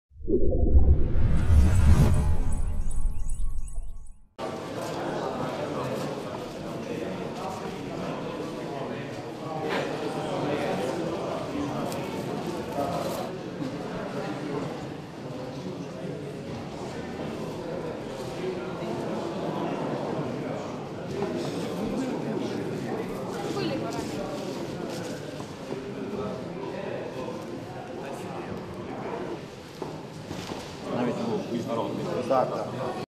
Без коментарів. Цими вихідними у Виноградові відбулась виставка елітних кролів.
Організатор - ГО «Елітна асоціація аматорів кролівництва та птахівництва Виноградівщини»